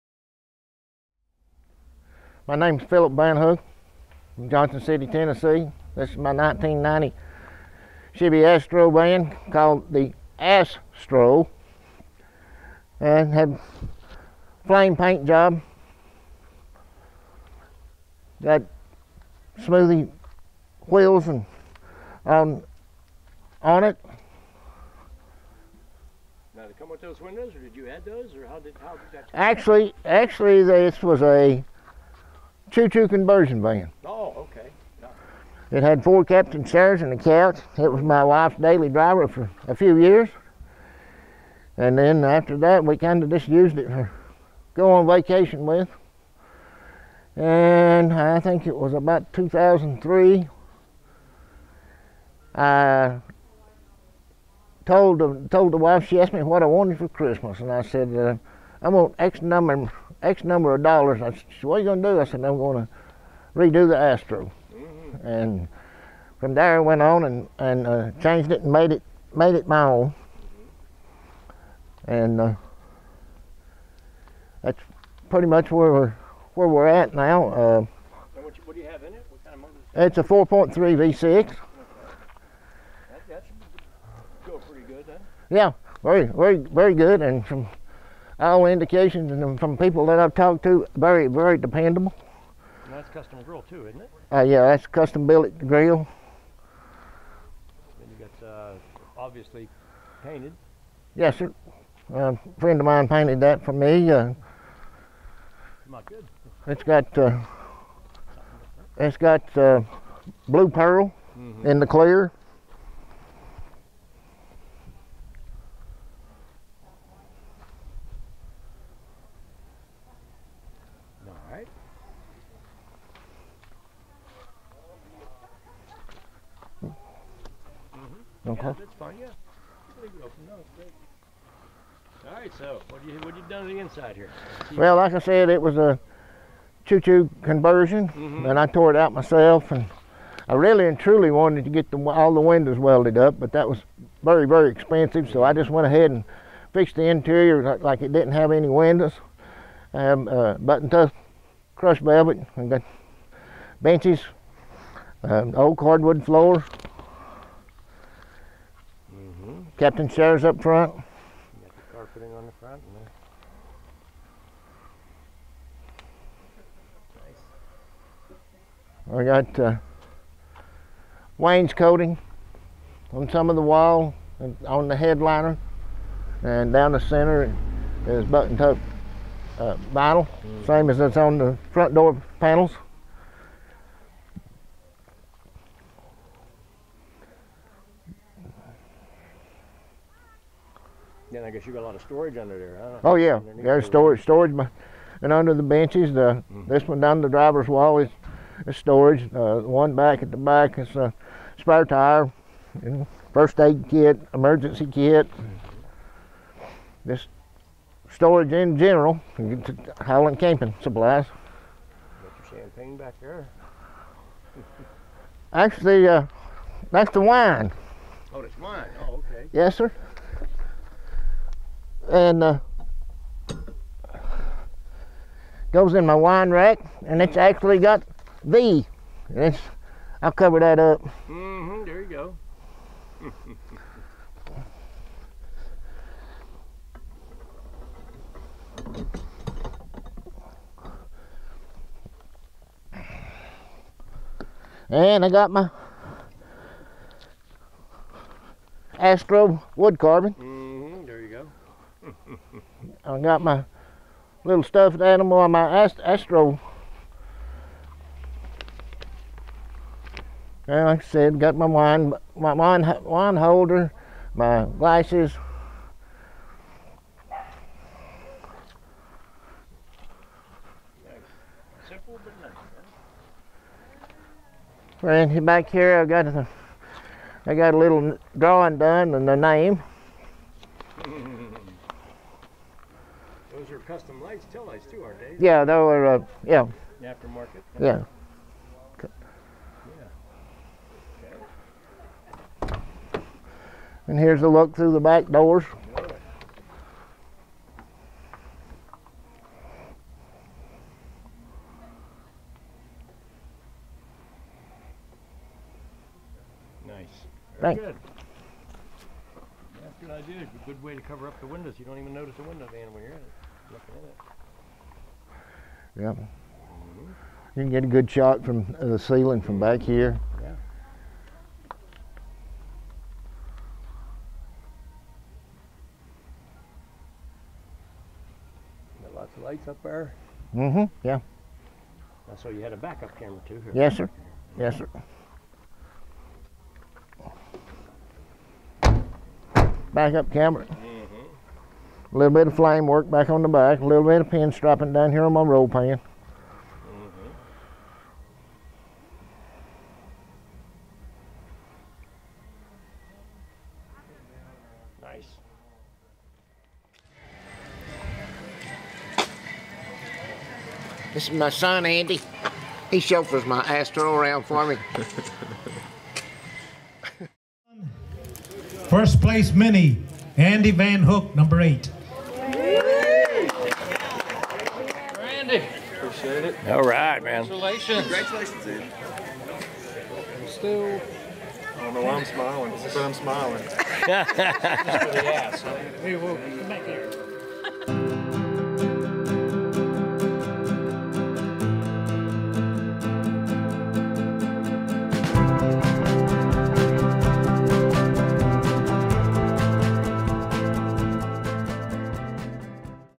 1990 Chevy Astro Mini Van. Custom.
Phillip Vanhook shows us around his 1990 Chevy Astro Custom Mini Van. He customized the interior to make it look like it had no windows. He calls it "Ass Stroh".  He is the same owner of the Y'All come Back Saloon Van. He had his son Andy show it at G.S.V.R.s Truck-in. He won Best Mini. Filmed by John Koterba & Jo at G.S.V.R.s Truck-in at The Flaming Arrow Campground.  Whittier, NC.